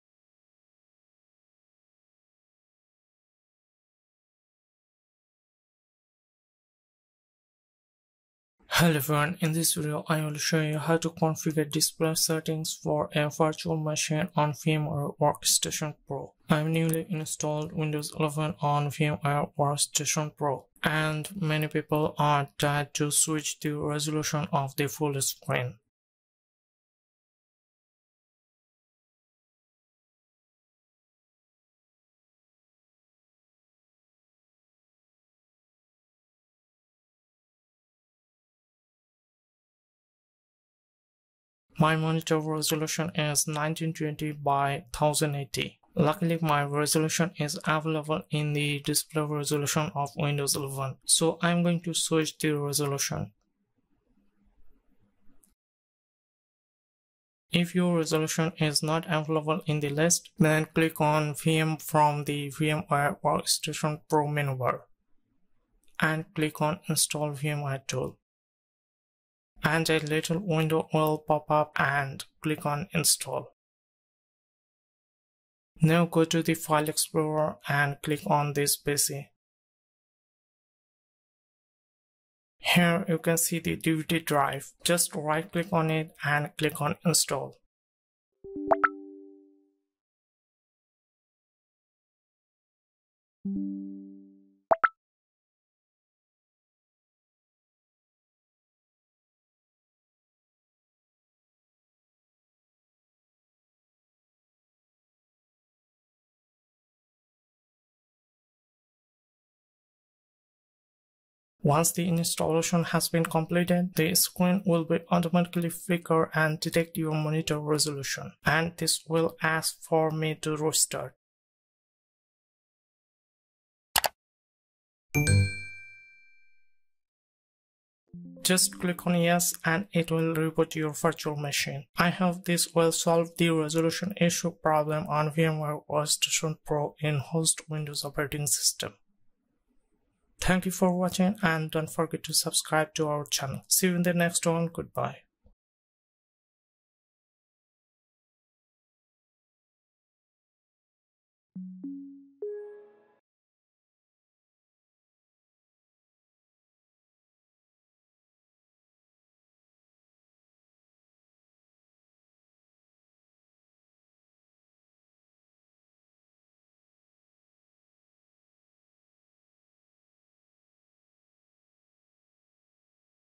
How to Configure Display Settings for a Virtual Machine on VMware Workstation Pro
How to Configure Display Settings for a Virtual Machine on VMware Workstation 16 Pro

I installed Windows 11on VMware, but now it won't allow me to set a resolution higher than 1152x864, while before the update, I was able to choose up to full monitor resolution (1920x1080). Reinstalled VMware Tools to solve the display resolution issue.